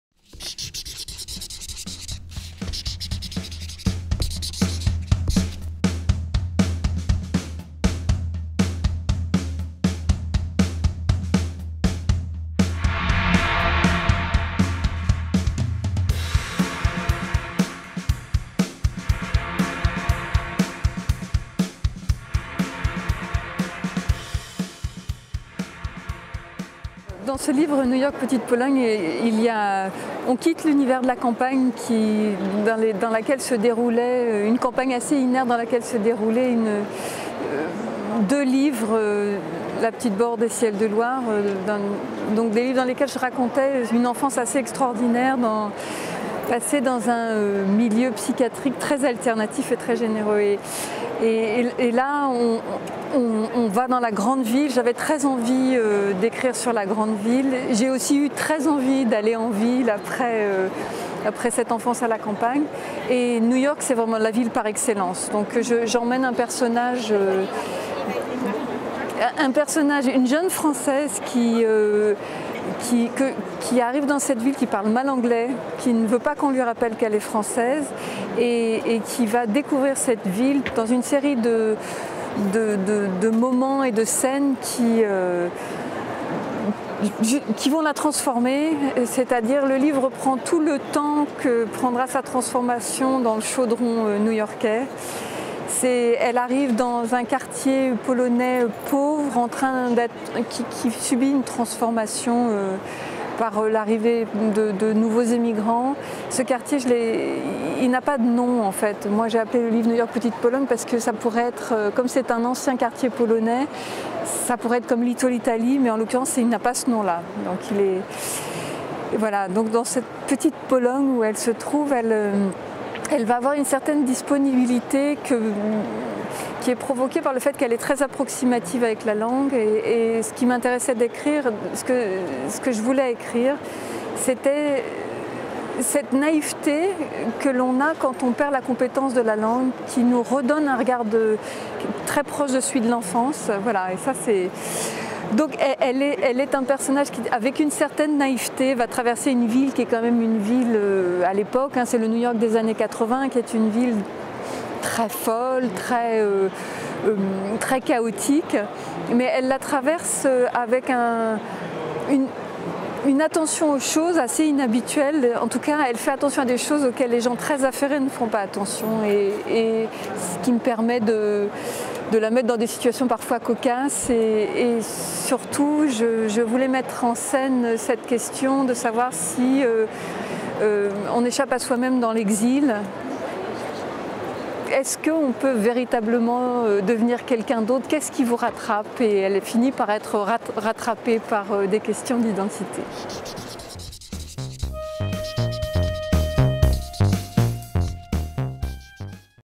Emmanuelle Guattari - New York, petite Pologne
À l'occasion du Salon du Livre de Paris 2015, Emmanuelle Guattari vous présente son ouvrage "New York, petite Pologne" aux éditions Mercure de France.